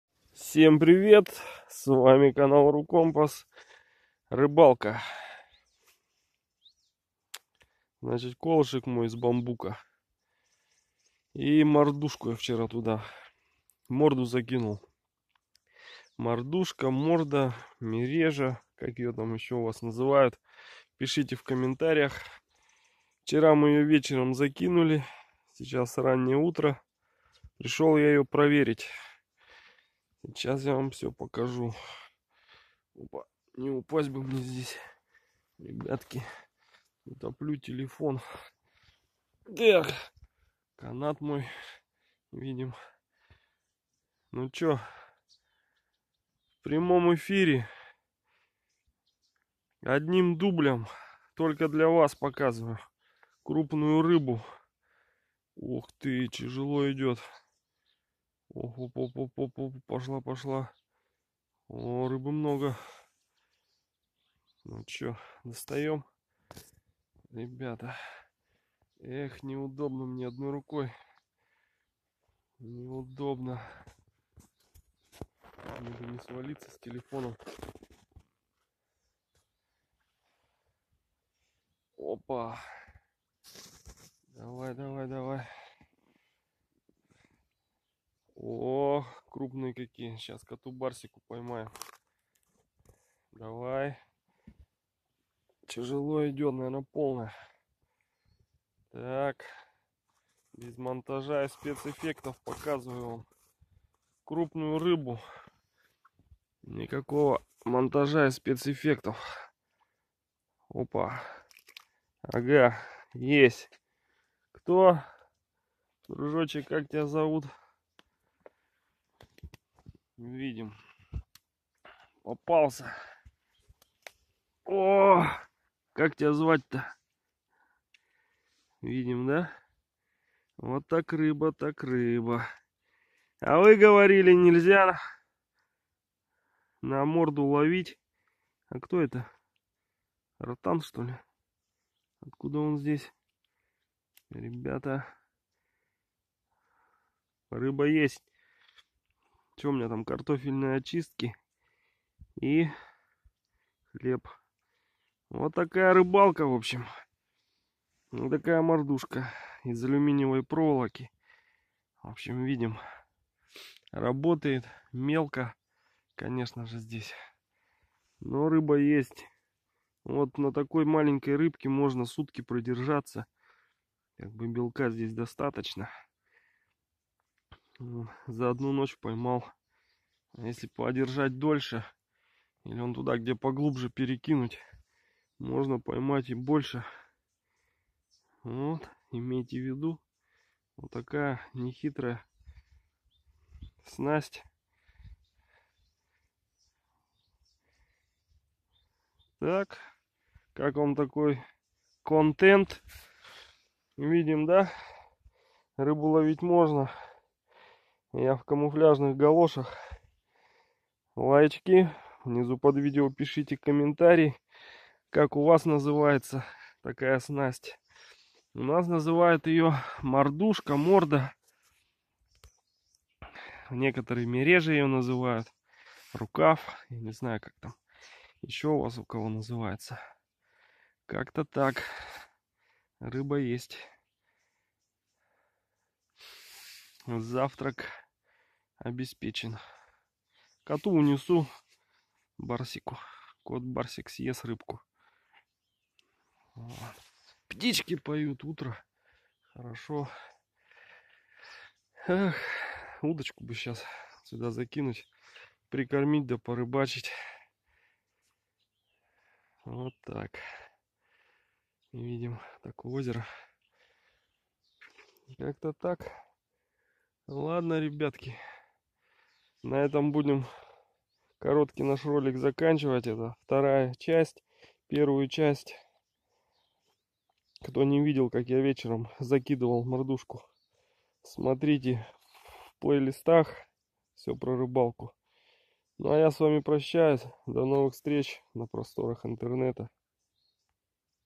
Рыбалка мордой, можно или нет поймать рыбу мордой
Морду используют для рыбалки вдоль берега. Перед тем как опустить ее в воду, внутрь кладут еловые ветки, которые приманивают рыбу, а отверстие верхнего конуса затыкают сеном. К основанию крепят грузы, чтобы ловушка не всплывала, а затем крепкой веревкой привязывают морду к кусту или вбитому в дно водоема колу.